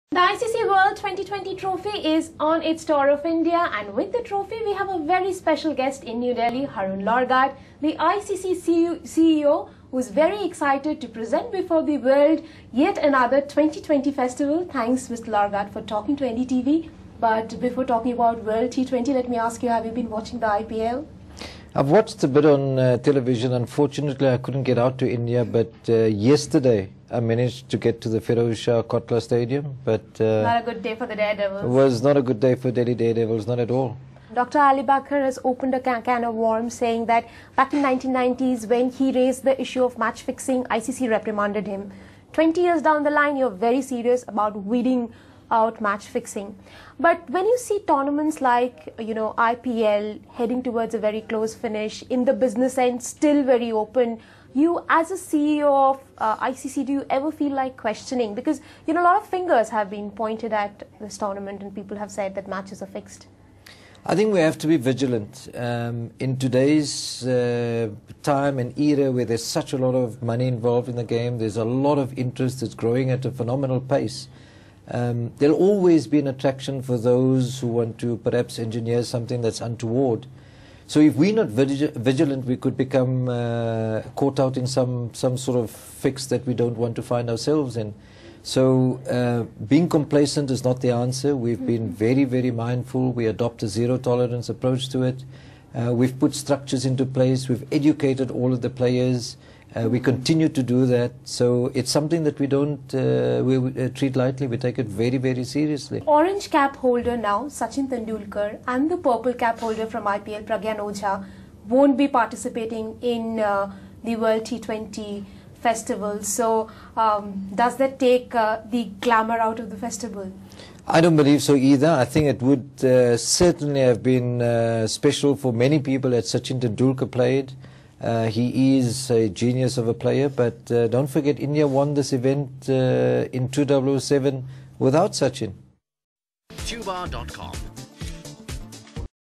IPL should be corruption free: ICC
ICC chief executive Haroon Lorgat said game administrators have to be vigilant enough to check corruption creeping into tournaments like the IPL.